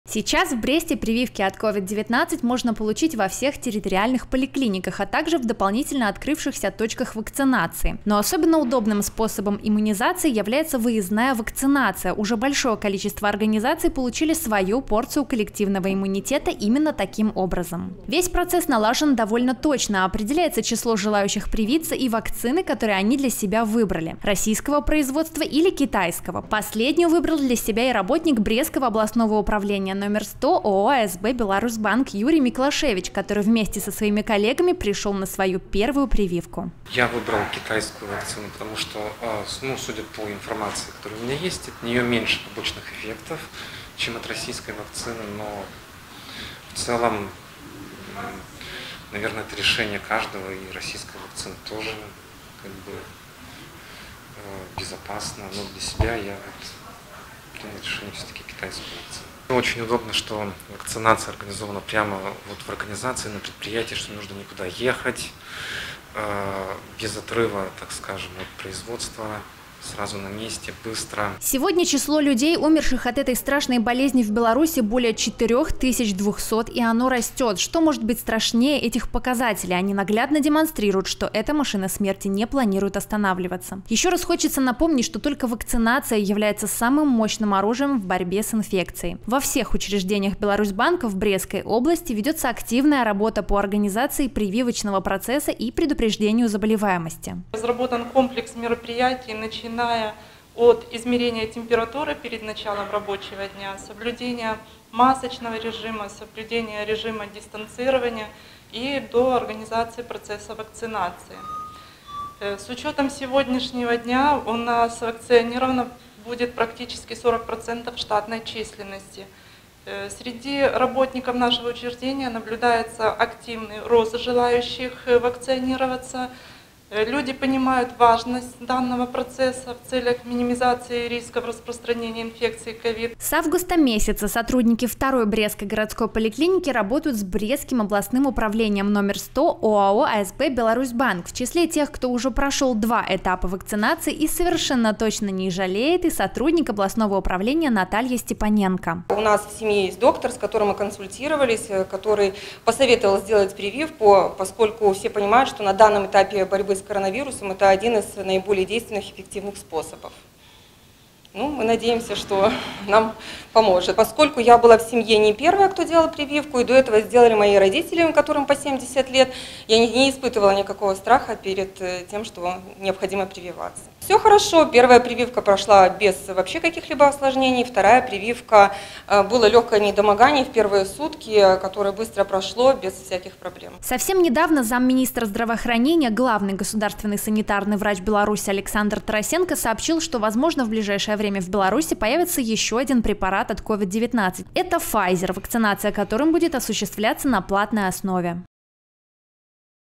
2021-10-06 г. Брест. И еще раз о вакцинации. Новости на Буг-ТВ.
И еще раз о вакцинации: филиал № 100 Брестское Областное Управление ОАО «АСБ Беларусбанк».
Эпидемиологическая ситуация в городе Бресте уже давно обрела нешуточные обороты. На протяжении последнего месяца   наблюдается значительный рост заболеваемости коронавирусной инфекцией. По состоянию на 5 октября в двух городских больницах госпитализировано 1300 пациентов с пневмонией, из которых 49 человек находятся в реанимации в тяжелом состоянии. Все 49 пациентов не были привиты.Такие удручающие показатели в очередной раз убеждают, что именно иммунизация всех слоёв населения остаётся наиболее эффективным способом борьбы с COVID-19. Как говорят врачи, только через коллективную вакцинацию может выработаться коллективный иммунитет. Прививочная компания  продолжается, продолжается и выездная вакцинация на предприятиях и организациях города Бреста. В этот раз выездная прививочная компания прошла в Филиале № 100 Брестское Областное Управление ОАО «АСБ Беларусбанк».